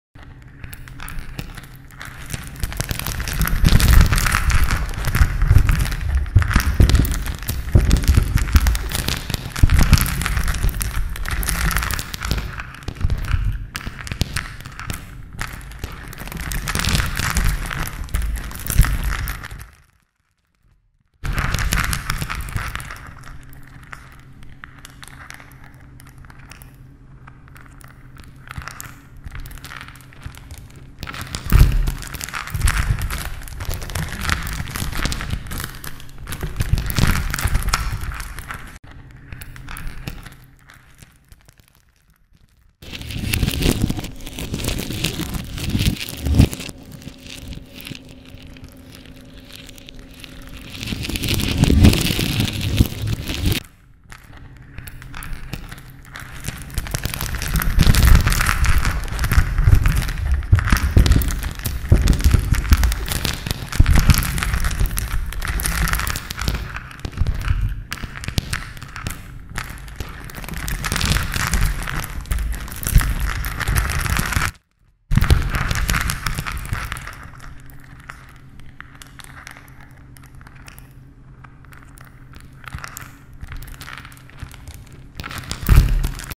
Fracture Blender Cycles Clay Render
Render time: 20 sec per frame on 2xGTX 780 Ti Zotac

Cell Fracture Add-on

4k Logo Animation PCZ Wydział Inżynierii Mechanicznej i Informatyki Politechniki Częstochowskiej Wimii Częstochowa University of Technology: